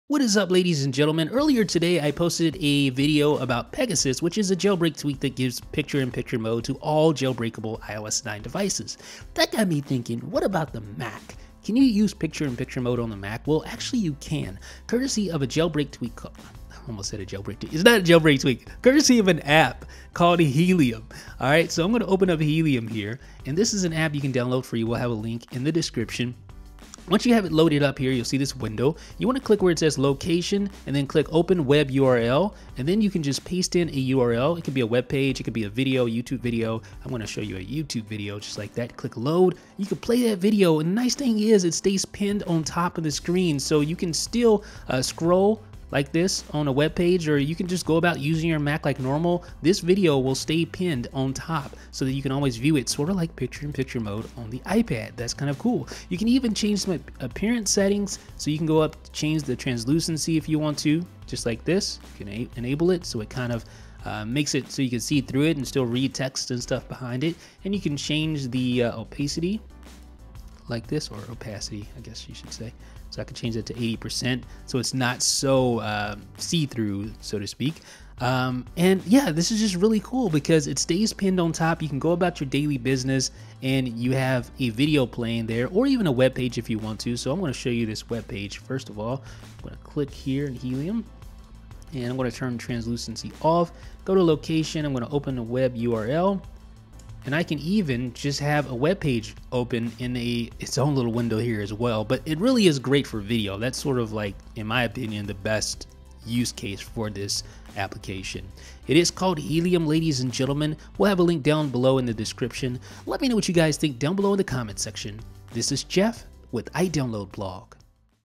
Helium brings Picture in Picture to Mac
Like iOS 9's Picture in Picture mode? Then check out Helium, it allows you to do similar things on Mac OS X.

About iDB